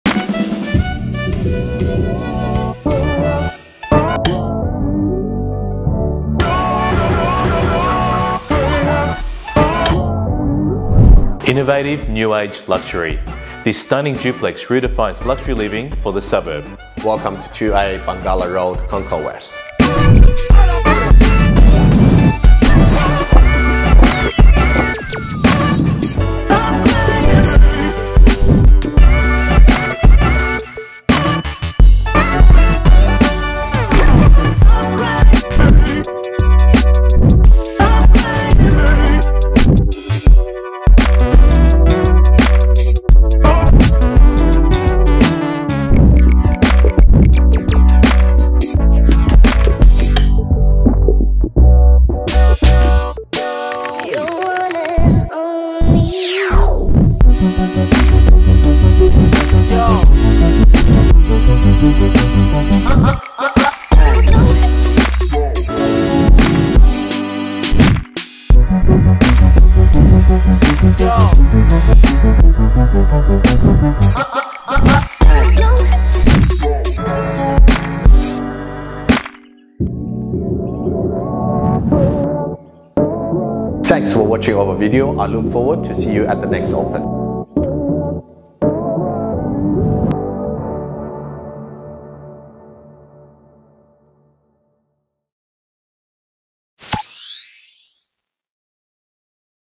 [Belle Property Strathfield] 2 Bangalla Road, Concord West
Striking architectural residence enjoys in/outdoor entertaining

A custom-appointed sanctuary of refinement and understated sophistication, this striking residence effortlessly unveils architectural flair, attention to detail and the finest of finishes. North-facing with an elegant sequence of living zones, its opulent interiors reveal a seamless in/outdoor living experience with four plush bedrooms. The property is located on a leafy tree-lined street just 850m from Concord West Station.

- Elegant interiors enhanced with floor-to-ceiling glass, tile and timber
- Private atrium plus an all-weather alfresco with an in-built kitchenette
- Child-friendly backyard boarded with beautifully landscaped gardens
- Designer kitchen with stone, integrated fridge and steel gas appliances
- Four double bedrooms with built-in robes plus a spacious home office
- Master includes a private balcony, dressing room and a double ensuite
- Ducted air-conditioning, in-built cabinetry, clean shadow line ceilings
- Exceptional readymade prospect within an area of continuous demand
- Internal access to the garage, off-street parking for another two vehicles
- Easy walk from amenities including shops, transport and quality schools